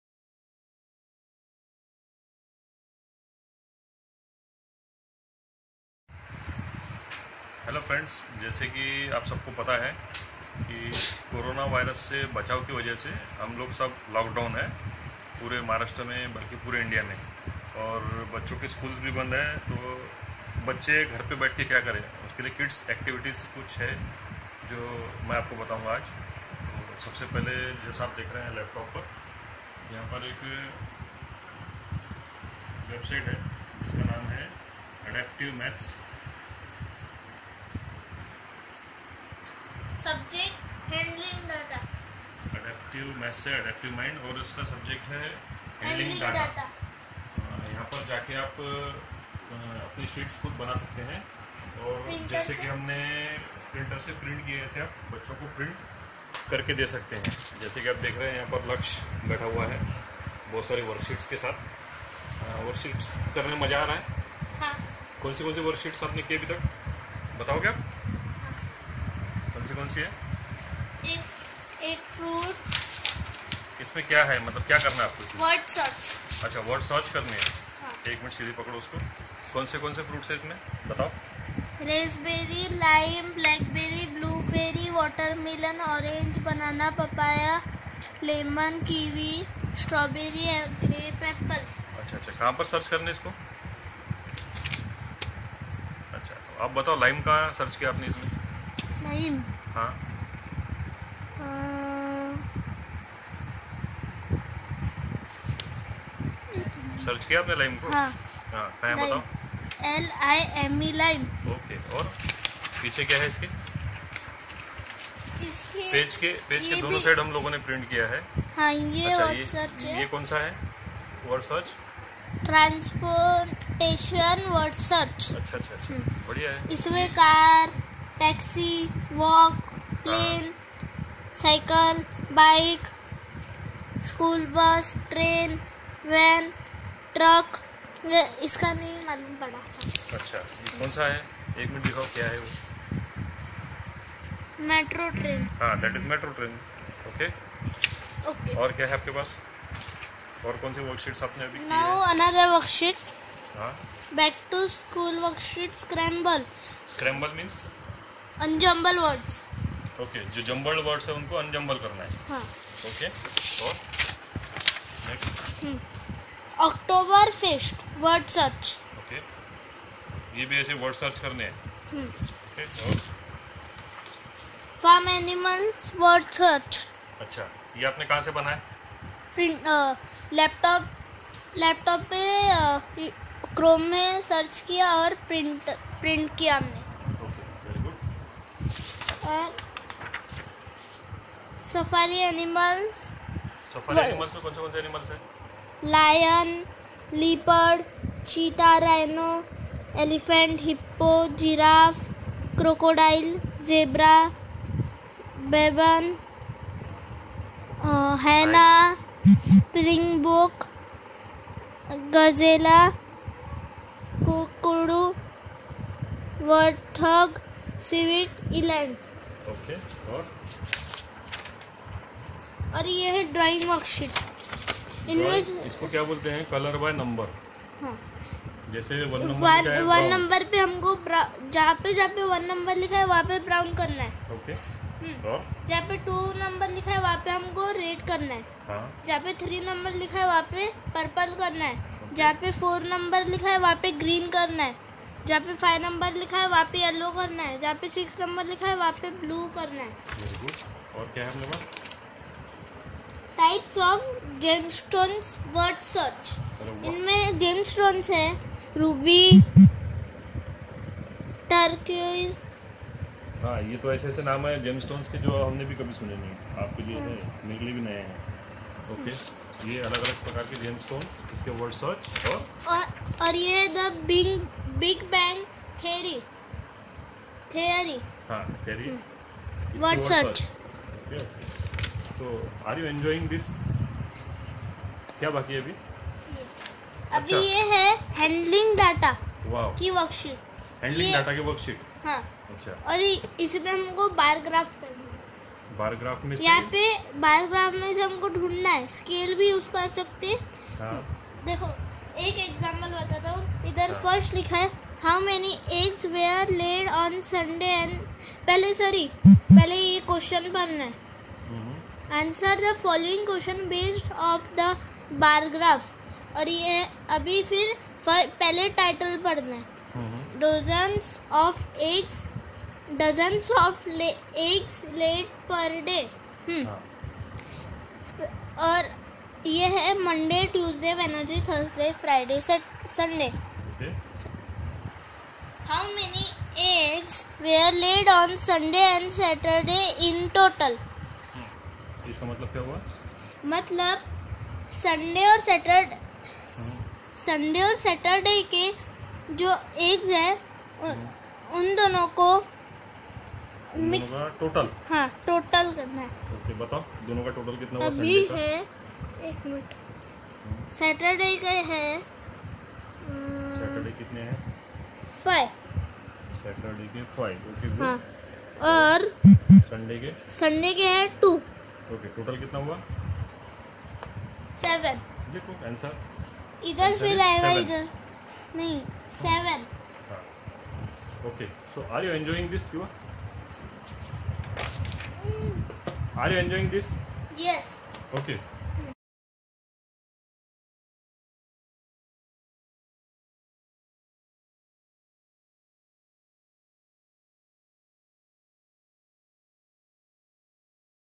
Kid's Home Activities Coronalockdown what is Yuvi doing?
This is a video about some activities your 4th grade,5th grade children can do during this lockdown period .

Please stay home and stay safe.